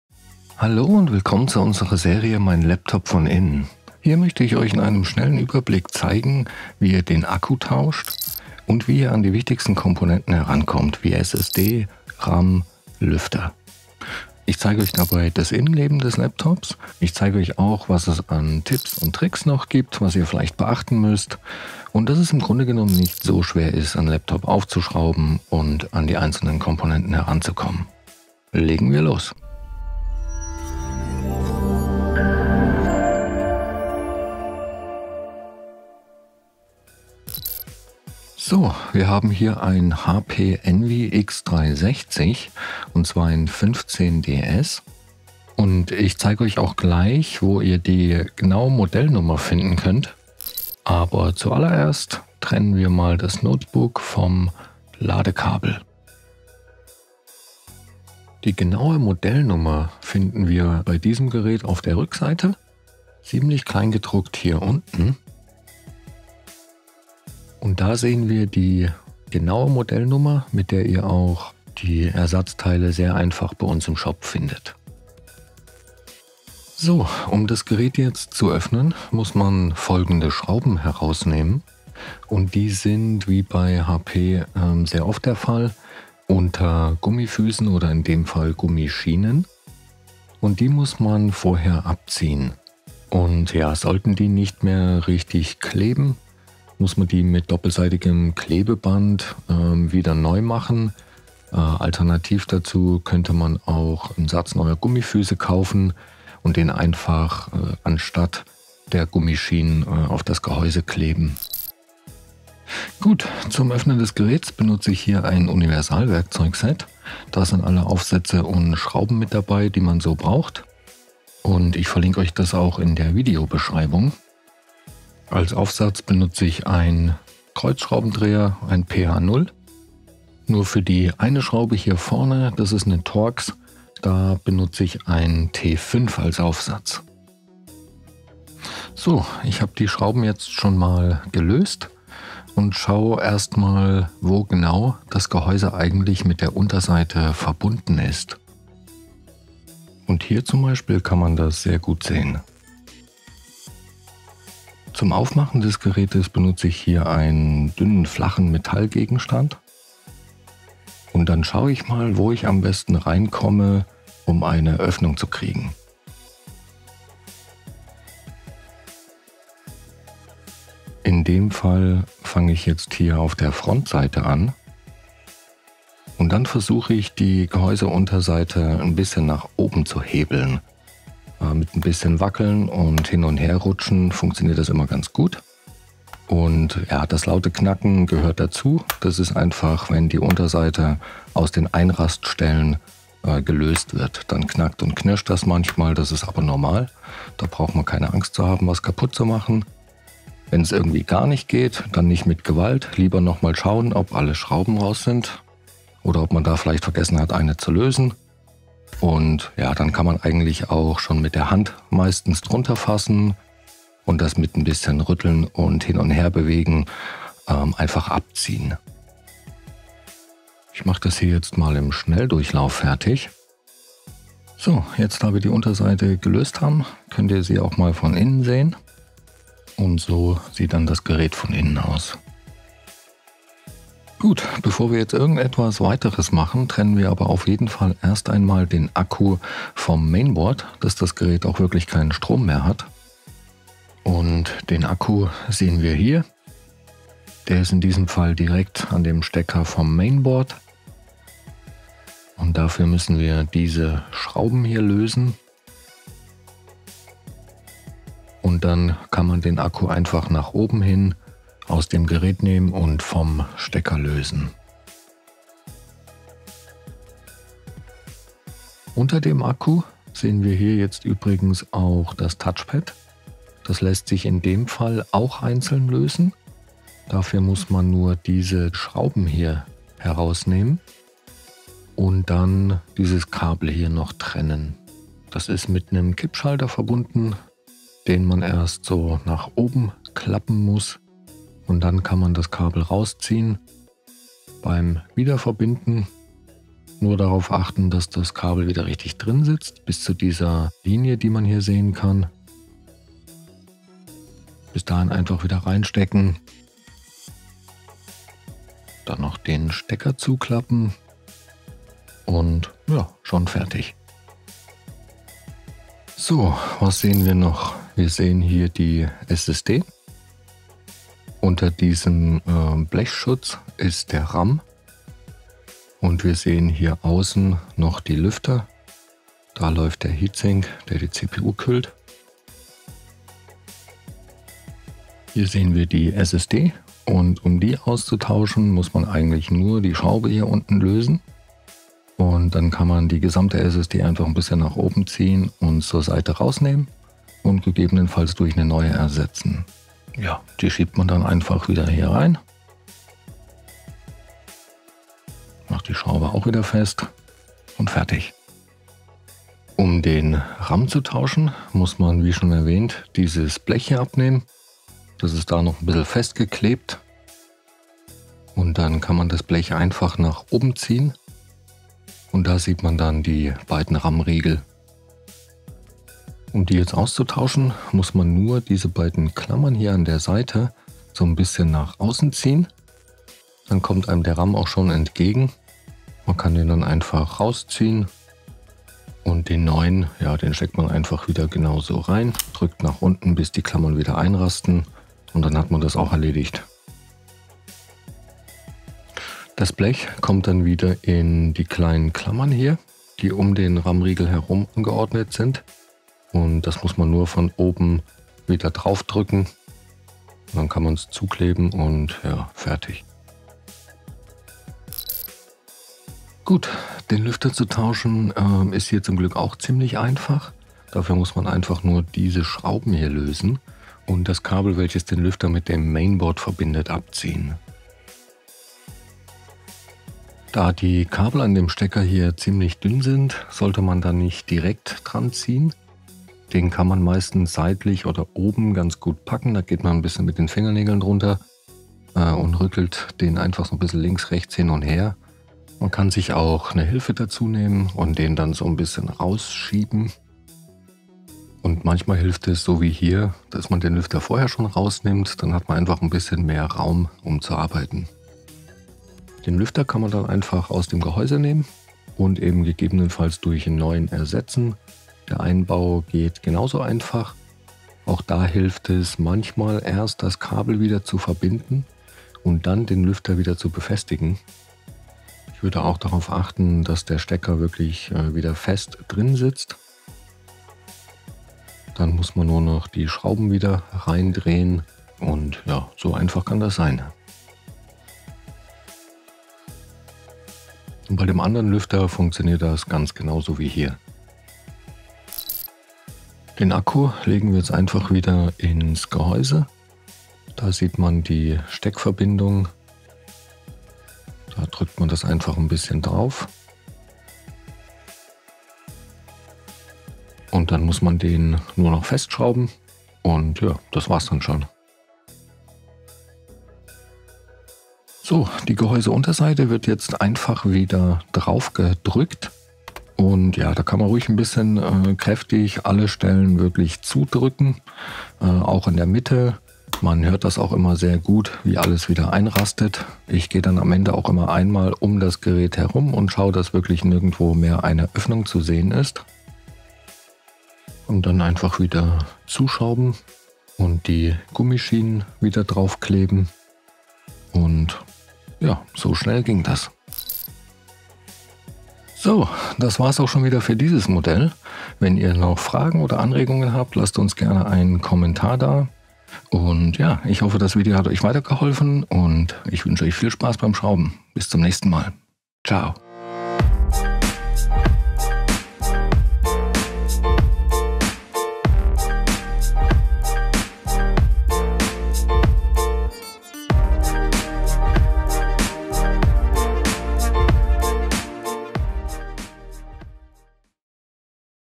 HP Envy x360 15-ds von Innen - RAM, SSD, Akku, Lüfter wechseln - Tutorial
Oft wirkt es bedrohlich das eigene Notebook aufzuschrauben und Ersatzteile selbst einzubauen. In diesem Video zeigt unser Experte wie ihr bei eurem HP Envy x360 einfach Komponenten wie Akku, SSD, RAM usw. finden könnt.

Gibt es doch Probleme könnt ihr euer Gerät auch für eine professionelle Reparatur anmelden

Wir sind jetzt auch auf Instagram!